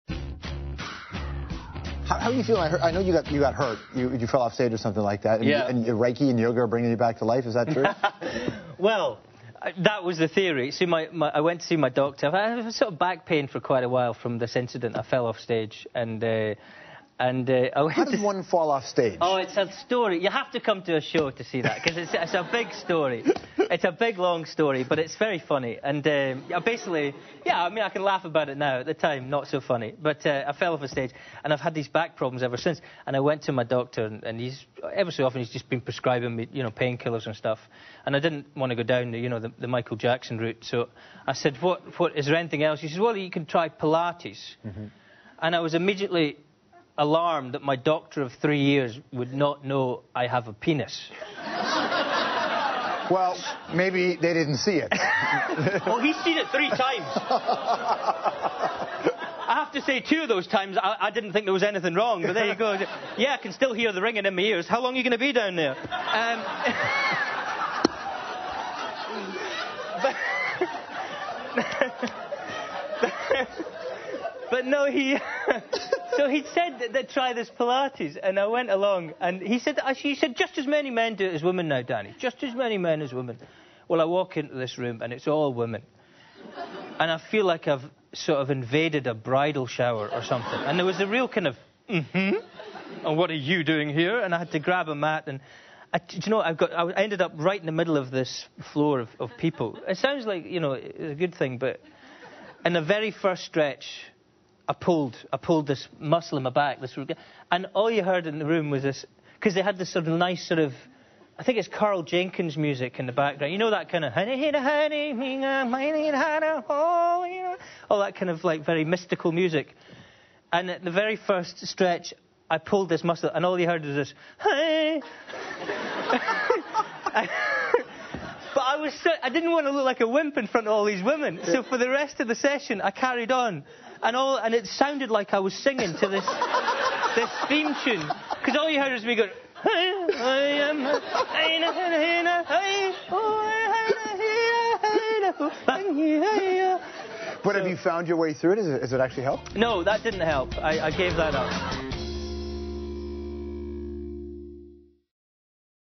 Danny Bhoy On His Road To Recovery
Comedian Danny Bhoy visits the show and talks about his road to recovery and pilates.